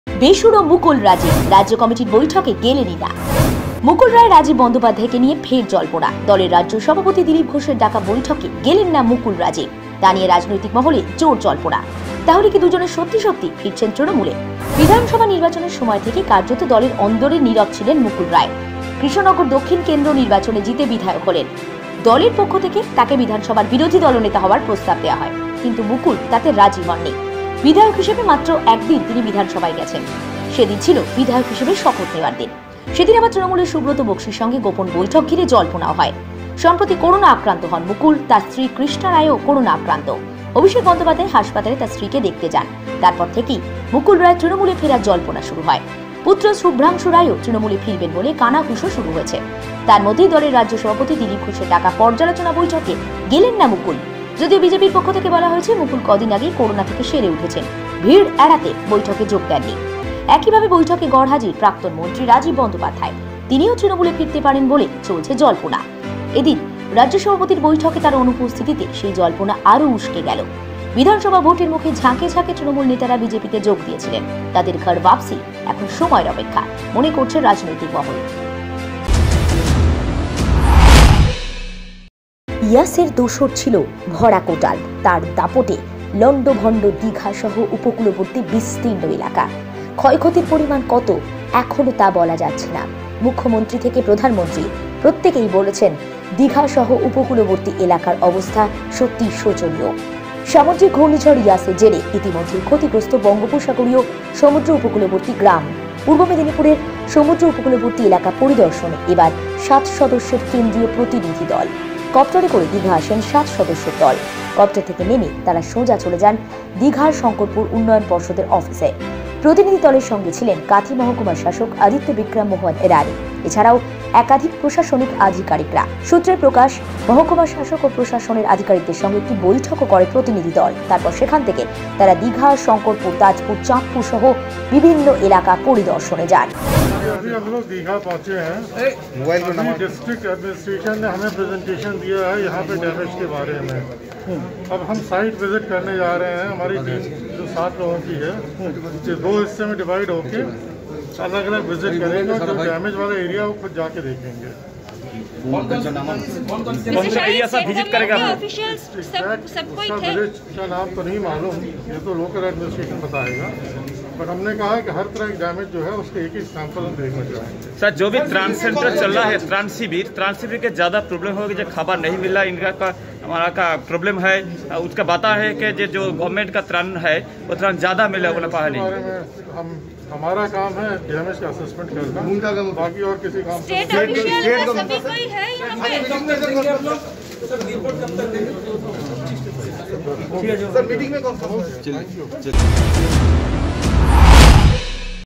বেসুরো মুকুল-রাজীব, রাজ্য কমিটির বৈঠকেই গেলেন না
Bengal Live:বেসুরো মুকুল-রাজীব, রাজ্য কমিটির বৈঠকেই গেলেন না